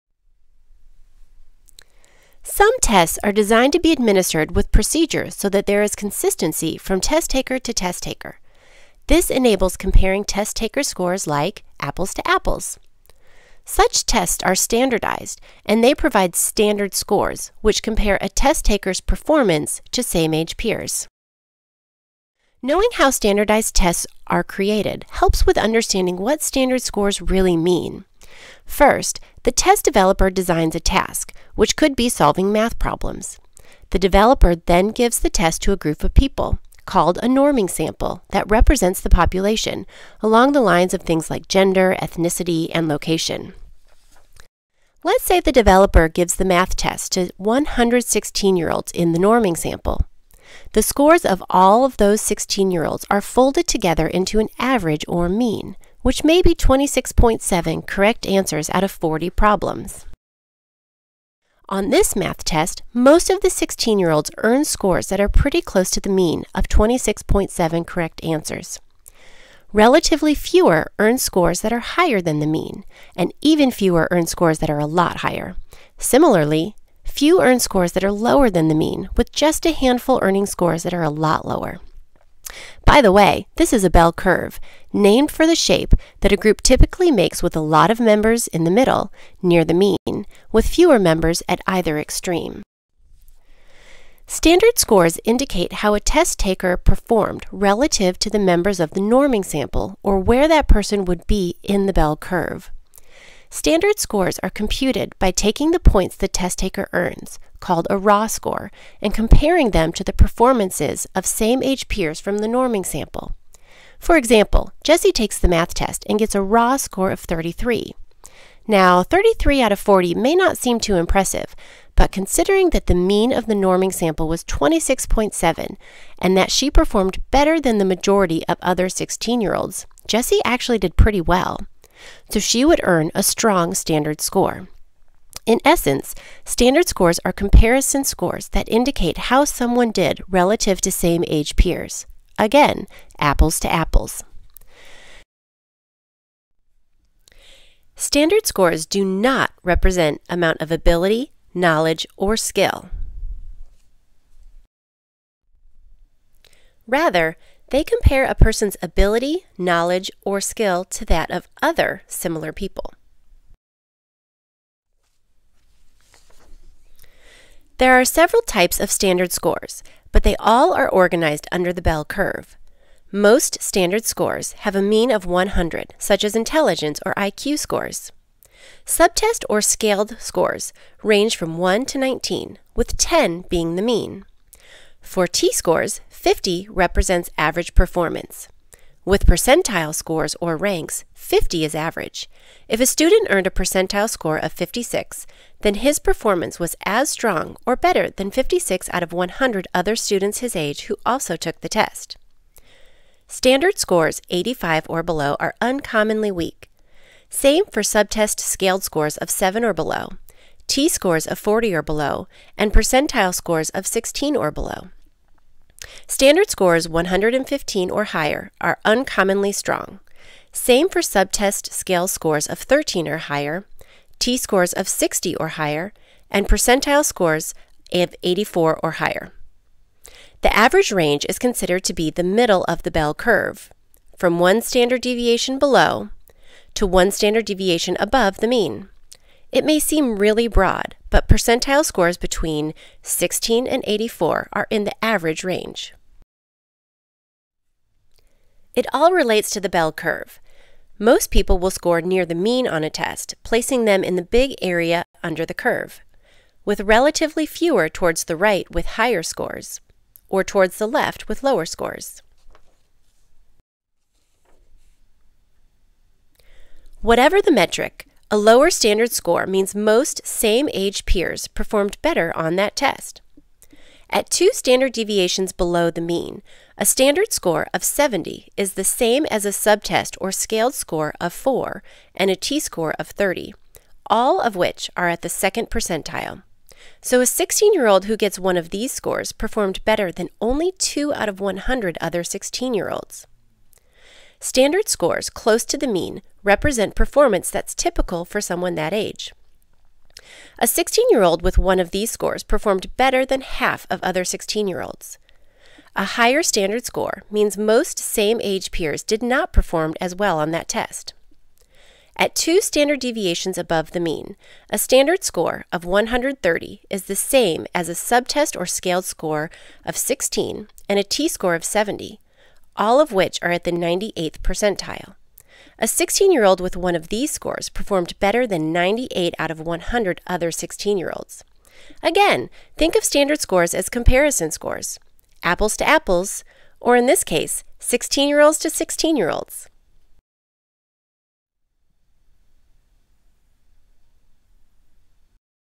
Standard Scores
Standard scores are comparison scores.  They allow a clinician to compare a test-taker's results to those of same-age peers.  Standard scores indicate where the test-taker would be under the bell curve.